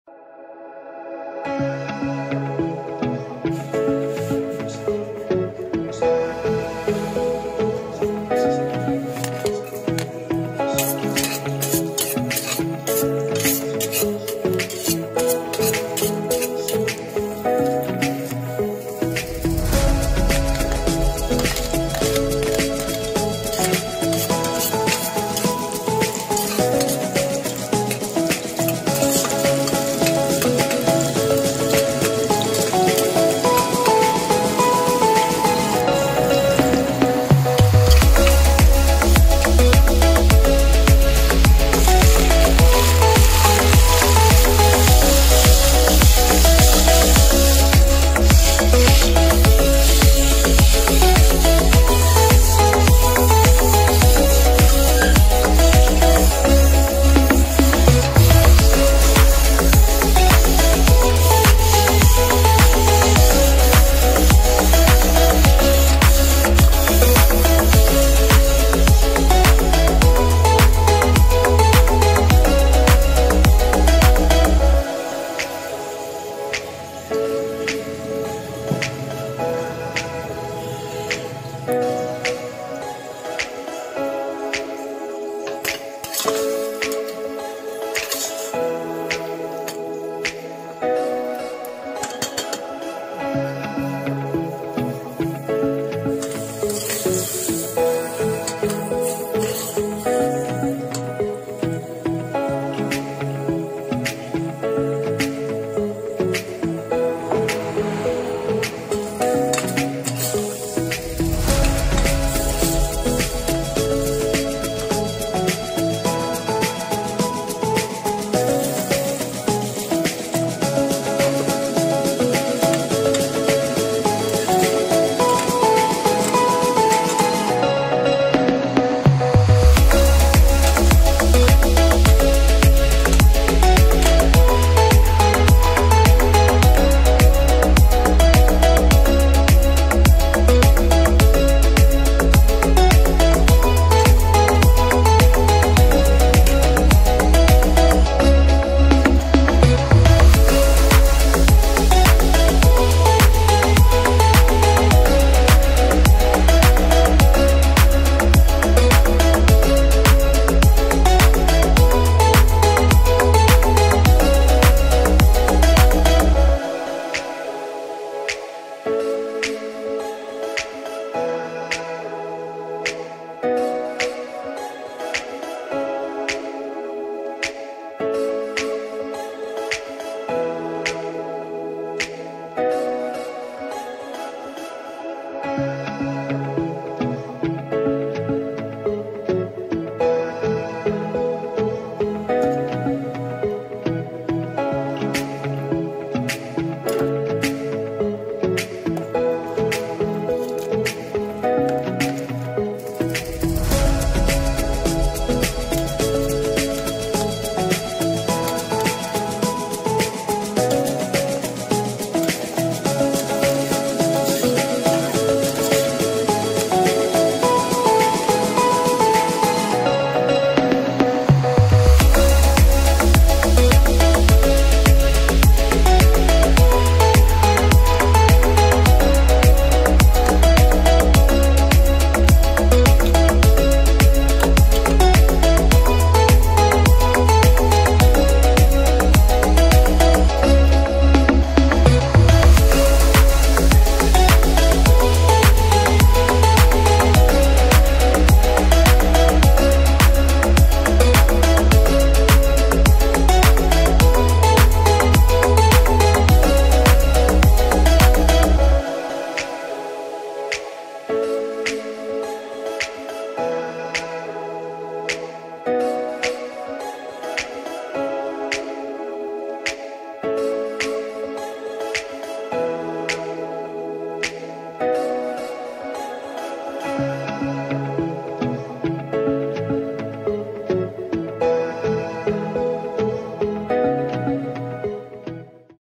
Misua with Kabatiti
Ingredients:

3 patola/Kabatiti sliced
1 chicken breast, cut into cubes
1 chicken cube
1 onion, chopped
4 cloves garlic, chopped
1 tsp ground black pepper
2 tbsp patis
1 small pack misua
1/2 tsp salt
4 cups water